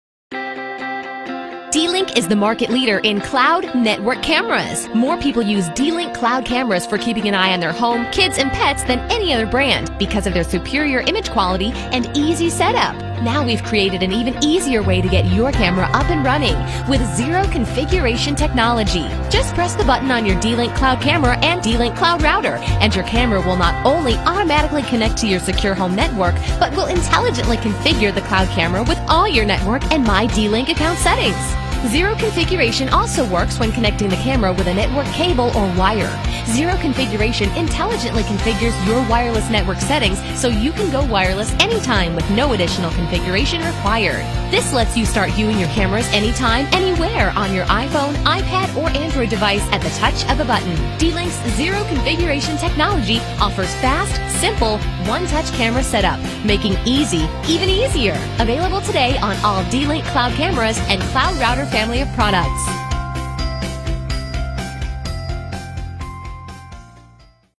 Zero Configuration D-Link - Características e funcionalidades
“Zero Configuration“ ou “Zero Configuração” é uma tecnologia exclusiva da D-Link que garante a instalação das câmeras da linha Cloud de forma 100% automática, eliminando totalmente qualquer processo de configuração, até mesmo os famosos cliques “next”.

SAC D-LINK:

Chat Online (D-Chat), das 8h às 20h, de segunda a sexta, na nossa página